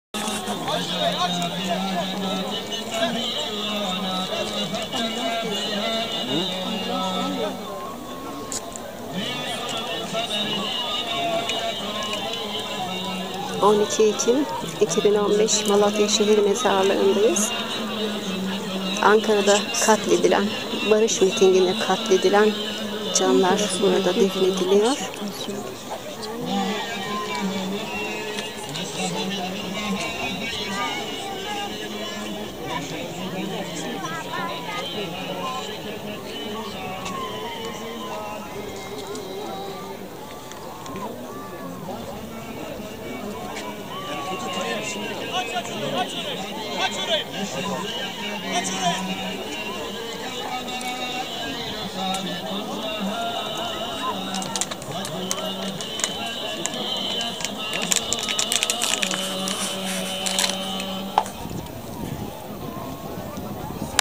Barışa gönül verenlerin güneşe onurlu yolculuğu-Sultan Kılıç
Şehir Mezarlığında Sezen Vurmaz, Gülbahar Aydeniz ve Gözde Aslan; dün defnedilen Eren Akın ve Mehmet Hayta'nın yanlarına defnedildiler. Malatya Şehir Mezarlığında12 Ekim 2015